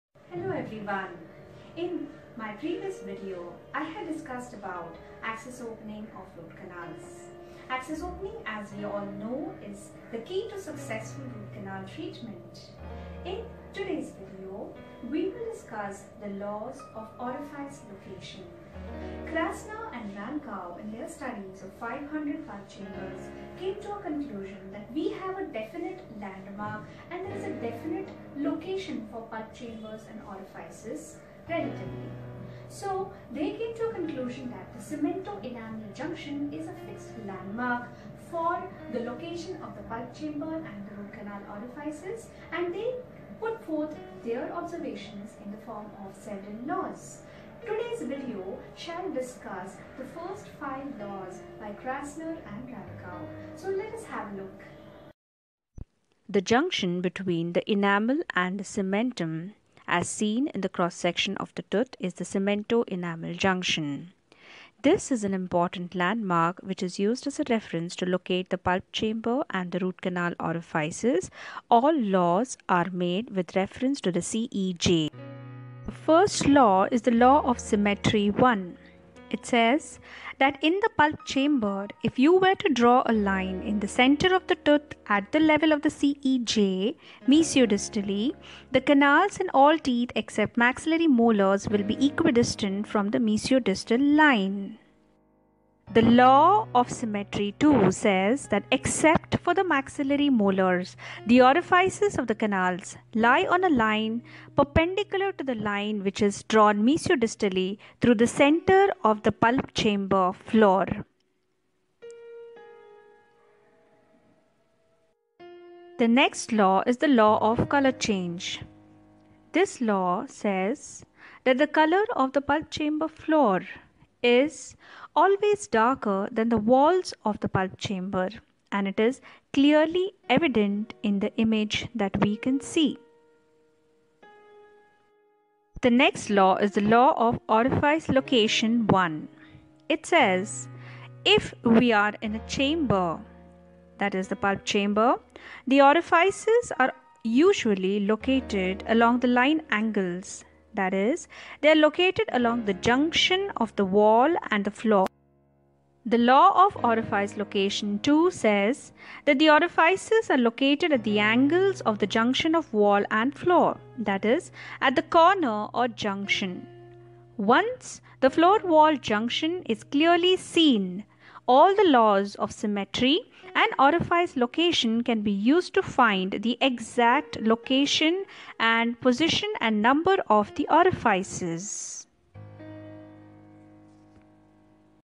ROOT CANAL ORIFICE LOCATION IN ENDODONTICS -1
Location of pulp chamber &orifices with Krasner & Rankow laws
The above laws were found to occur in 95% of the teeth
examined. Five percent of mandibular second and third
molars did not conform to these laws because of the
presence of C-shaped canal anatomy.